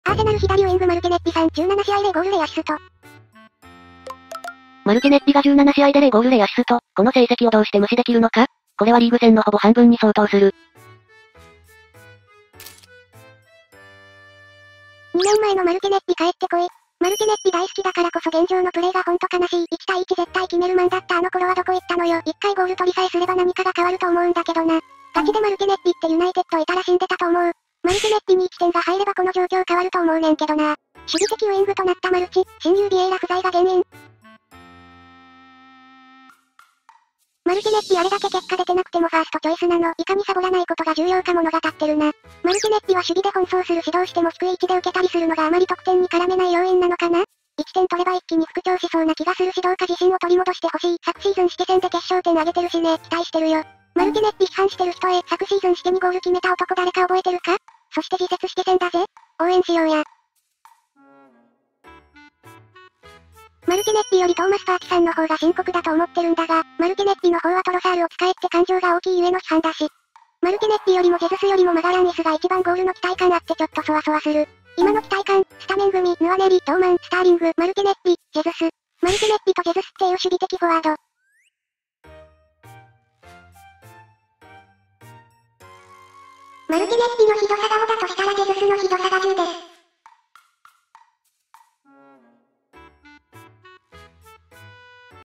【悲報】アーセナル左WGマルティネッリさん、17試合0ゴール0アシスト…
こんなに遠ざかってたか